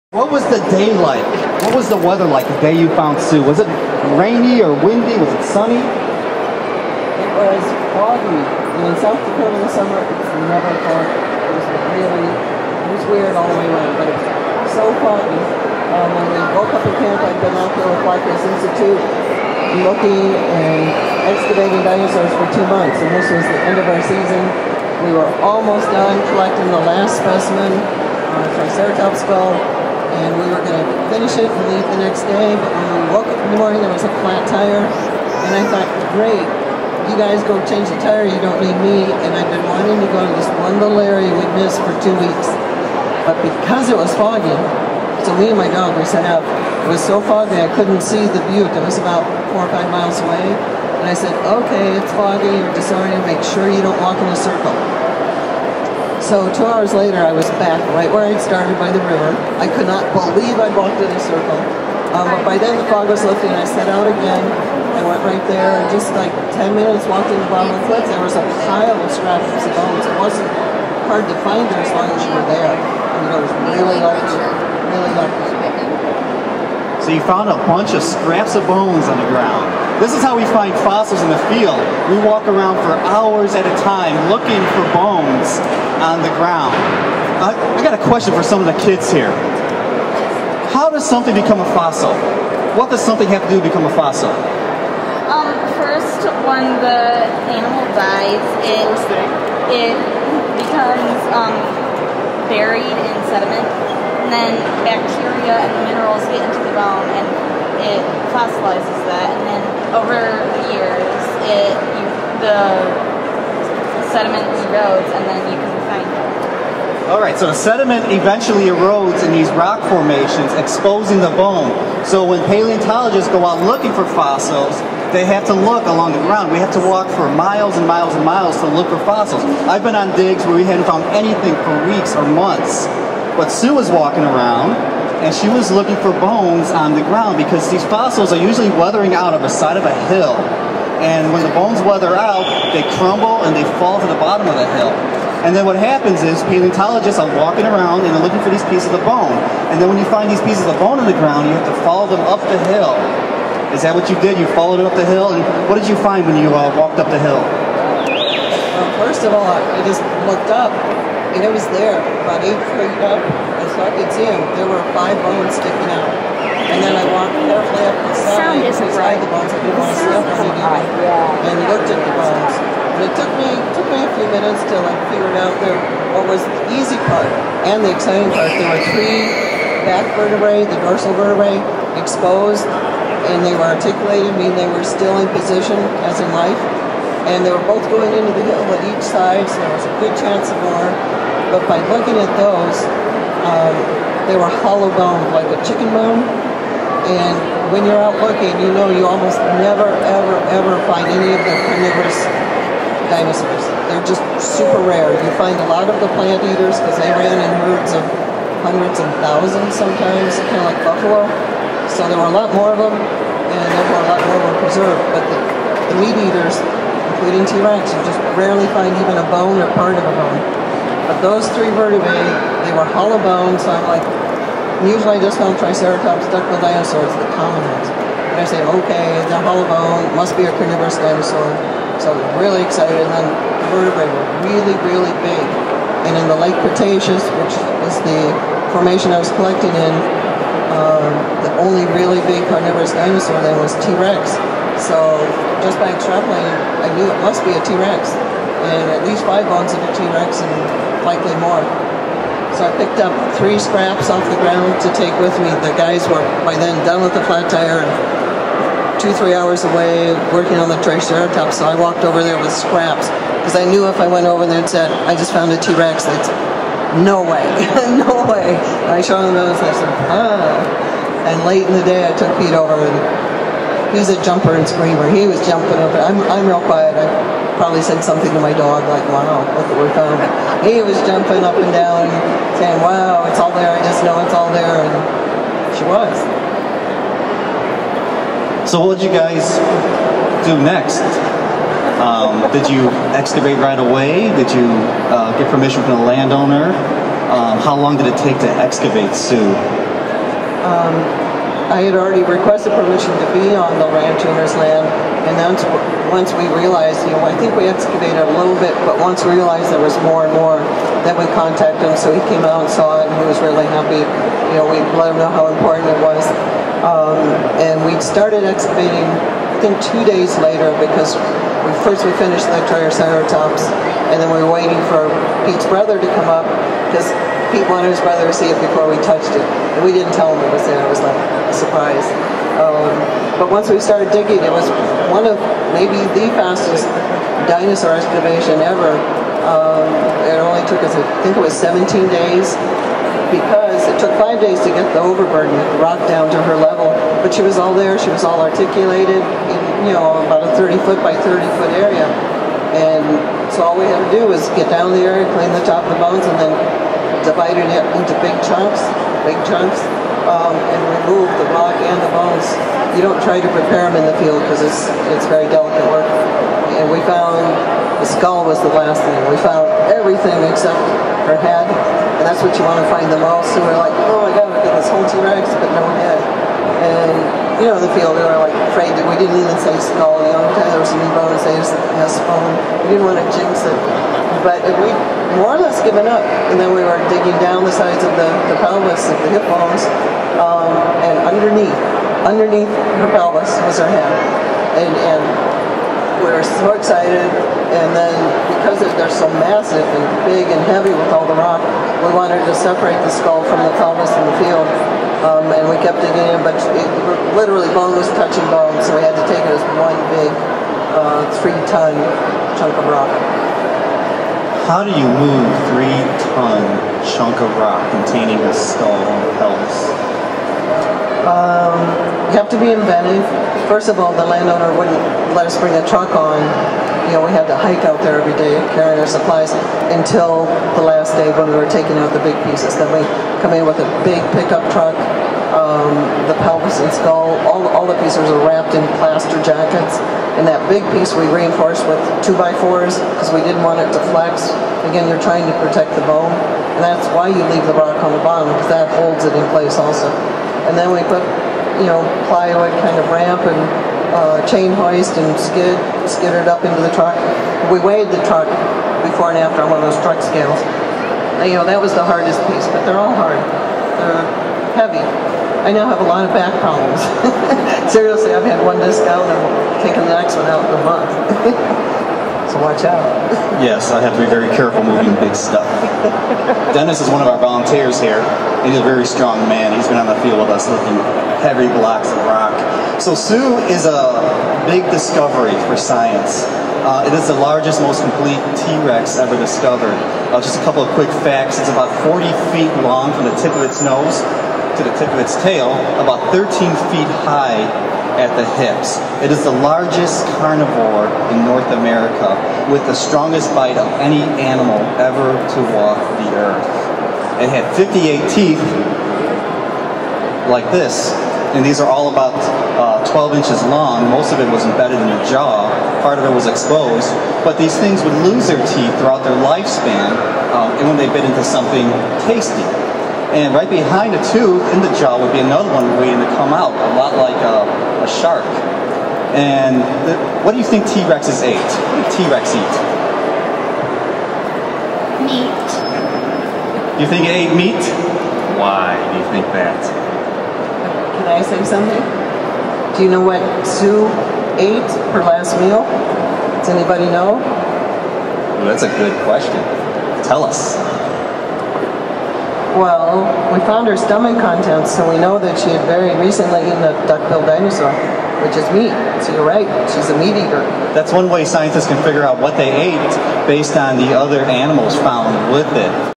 SUE School at the Field Museum
Sue Hendrickson and Jim Holstein answer questions about Sue the T-Rex on May 29, 2010 at The Field Museum in Chicago, IL during the celebration of the 10th anniversary of the discovery of Sue, the most complete T-rex skeleton ever found.